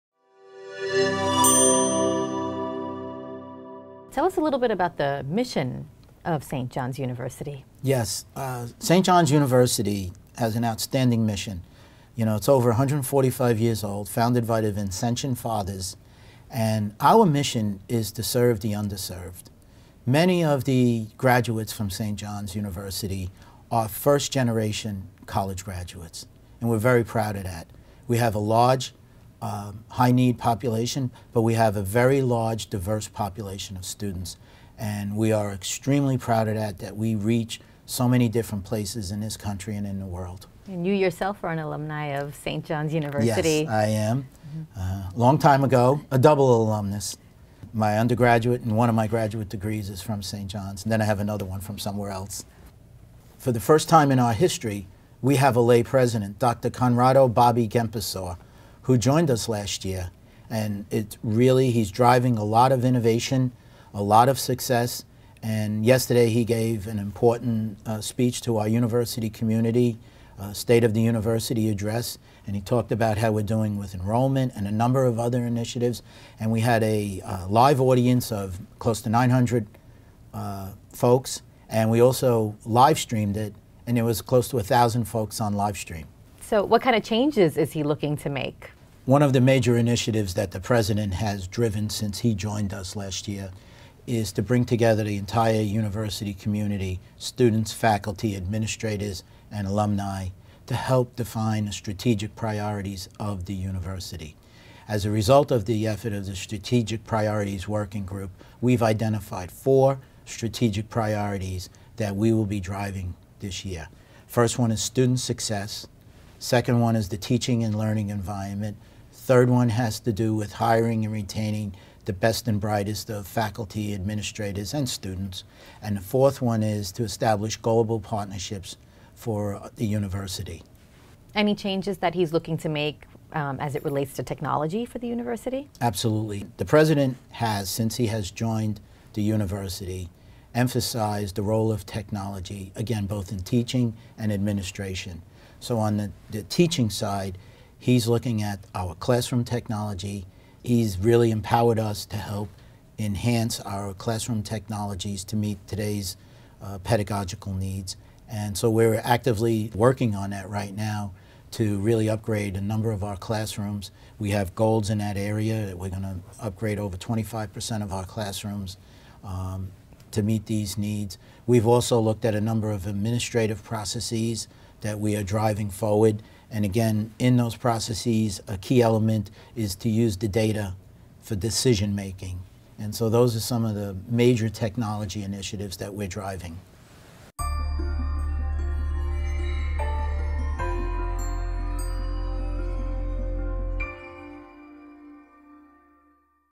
What is the mission of St. John's University? by Joseph Tufano, CIO at St. John’s University
A double alumnus of St. John’s, Joseph Tufano joined the University in 2002 and is currently the Vice President and Chief Information Officer of Information Technology. During his tenure, he has transformed the culture of the academic community, driving its commitment to the use of technology in every facet of campus life.
He has received individual recognitions for his work at St.
John’s, which include:
• Computerworld’s 2008 Premier 100 IT Leaders, presented to IT executives who effectively executed IT strategies in their organizations
• CIO Magazine’s 2008 CIO 100 Award, presented to technology leaders who added new IT value in innovative ways 2008 and 2012 Program Judge for Computerworld’s Honors Program

Mr. Tufano has more than 30 years experience in the information technology industry. Prior to joining St. John’s, he held executive positions at Prudential Financial, Inc., and MetLife. Mr. Tufano earned a B.A. and M.A. in Mathematics from St. John’s University and an M.S. in Computer Science from Polytechnic University. Mr. Tufano and his wife, reside on Long Island.

Joseph Tufano talks about the mission of St. John's University that is to serve the underserved. He also talks about the University's new president and his strategic priorities.